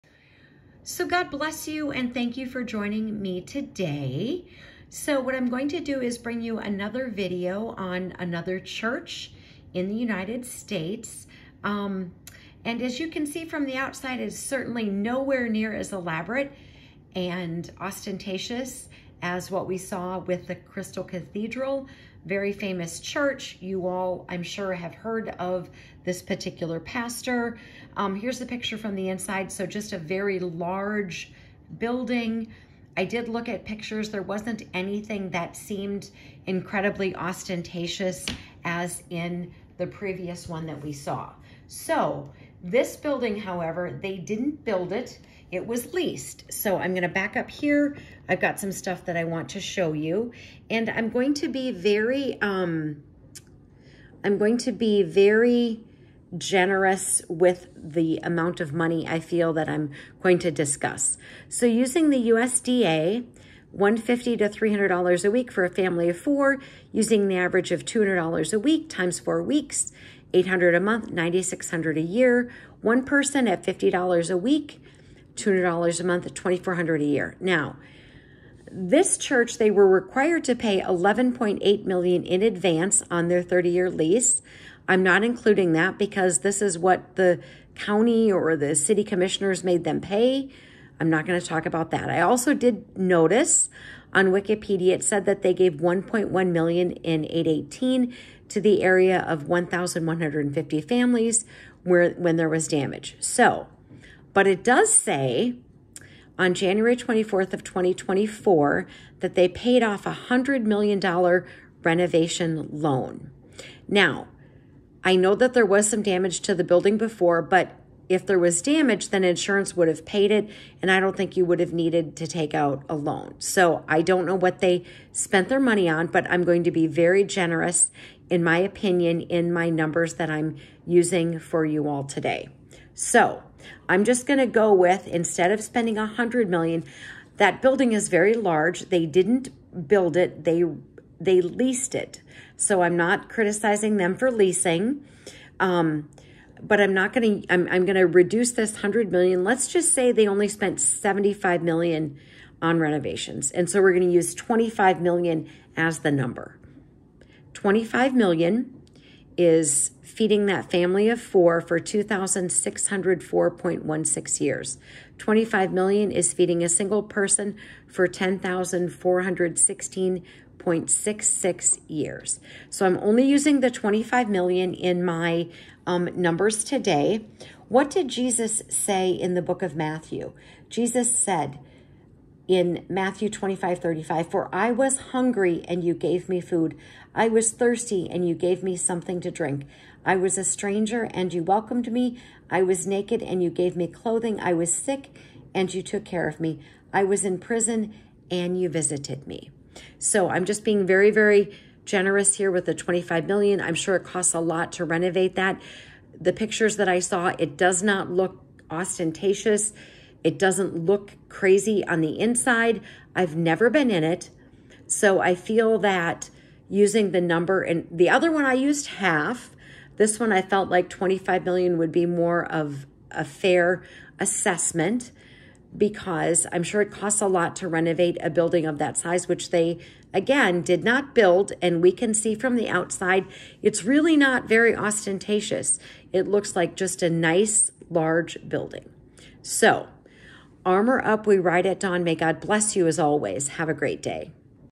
Our messed up church
Are we using money wisely instead of feeding the homeless?  (Matt 25 v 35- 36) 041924.